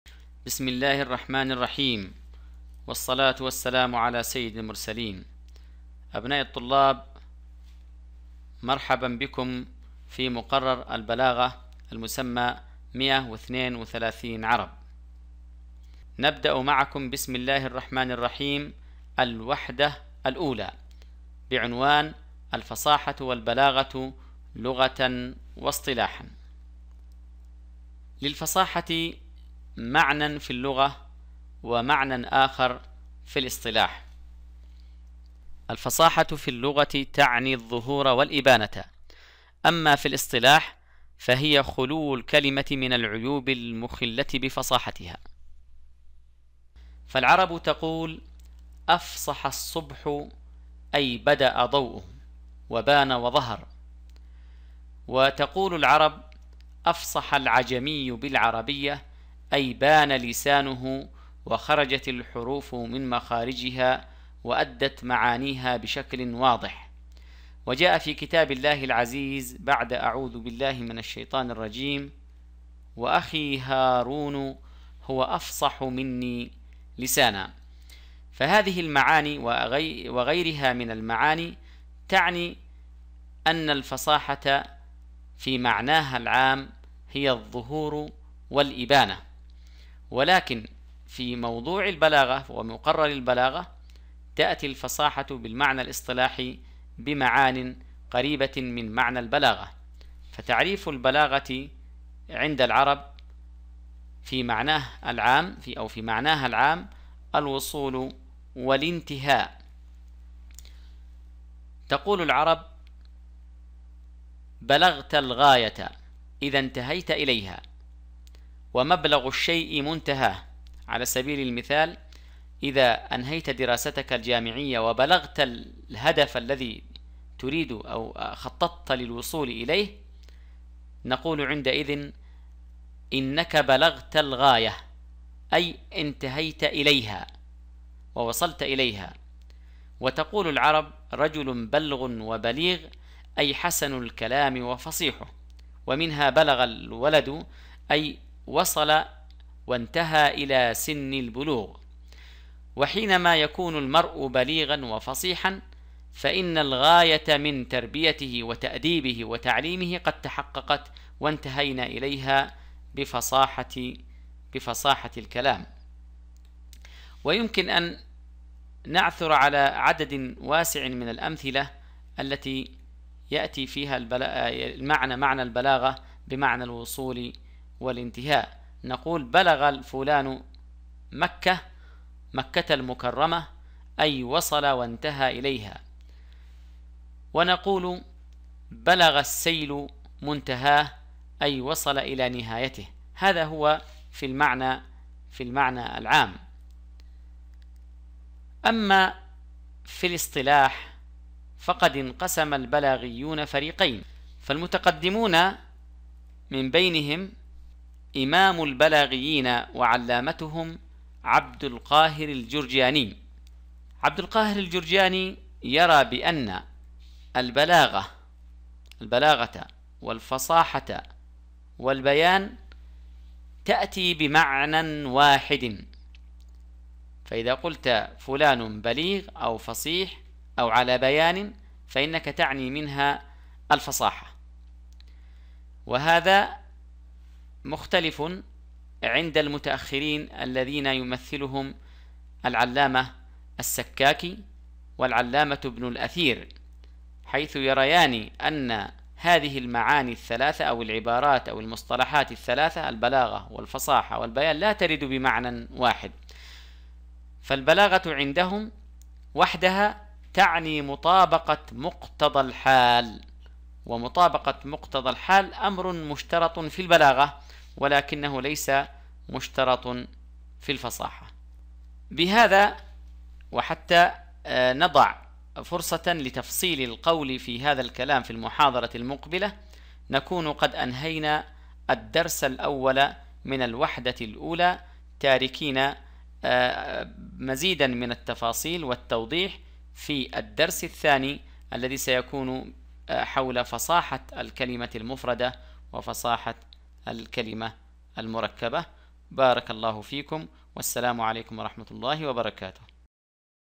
الوحدة الأولى: الفصاحه والبلاغة لغه واصطلاحا
مفهوم الفصاحه لغه واصطلاحاً , البلاغه لغه واصطلاحاً